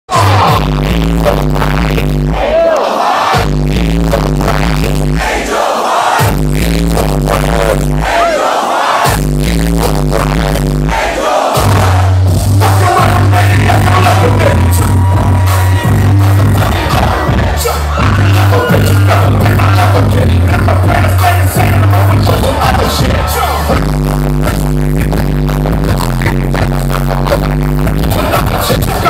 TECH N9NE
Tech N9ne's Independent Powerhouse Tour 2016. The Catalyst Club. Santa Cruz, CA